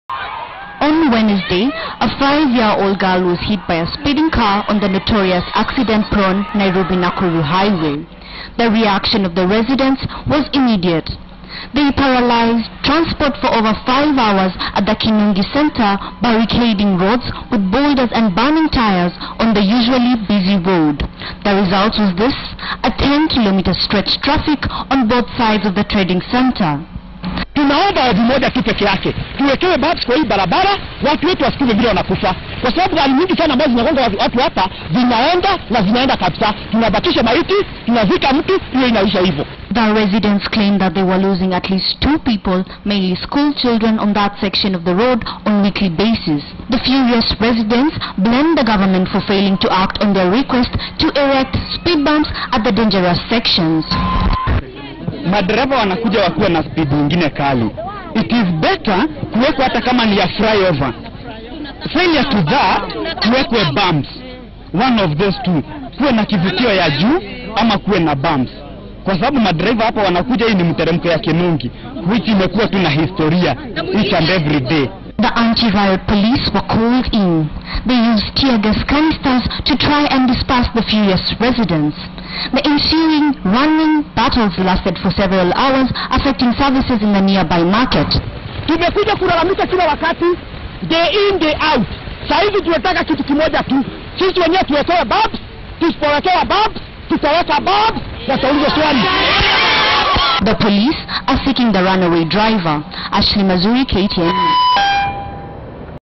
Transport along Nairobi-Nakuru paralyzed after 5 year old minor was hit and killed by speeding car
Transport along the Nairobi-Nakuru highway in Naivasha was paralyzed for over five hours on Wednesday night and Thursday after a five year old minor was hit and killed by a speeding car---the irate residents of Kinungi center using boulders and burning tires barricaded the busy road leading to a massive jam that stretched for over 10kms on either sides of the trading center.